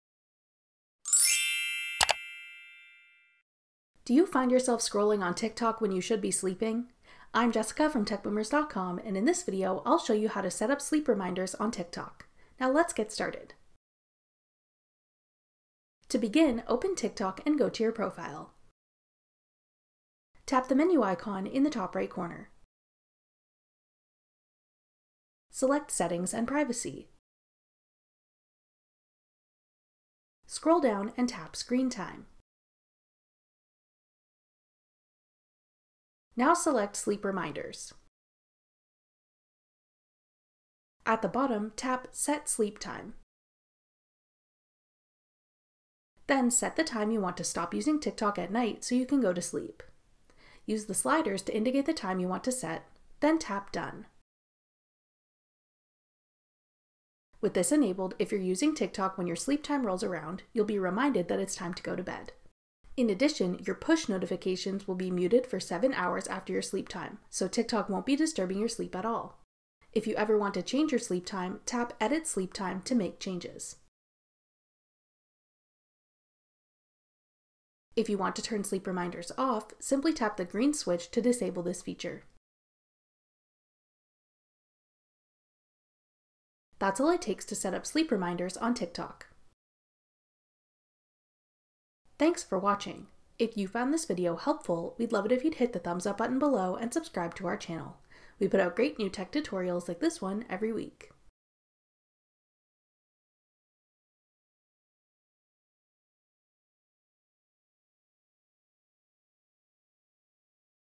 How to Set Up TikTok Sleep Reminders
Do you find yourself scrolling on TikTok when you should be sleeping? Watch this video to learn how to set up sleep reminders on TikTok!

To begin, open TikTok and go to your profile. Tap the menu icon in the top right corner. Select Settings and Privacy. Scroll down and tap Screen Time. Now select Sleep Reminders. At the bottom, tap Set Sleep Time. Then set the time you want to stop using TikTok at night so you can go to sleep. Use the sliders to indicate the time you want to set, then tap Done. With this enabled, if you’re using TikTok when your sleep time rolls around, you’ll be reminded that it’s time to go to bed. In addition, your push notifications will be muted for 7 hours after your sleep time, so TikTok won’t be disturbing your sleep at all.

If you ever want to change your sleep time, tap Edit Sleep Time to make changes. If you want to turn sleep reminders off, simply tap the green switch to disable this feature. That’s all it takes to set up sleep reminders on TikTok.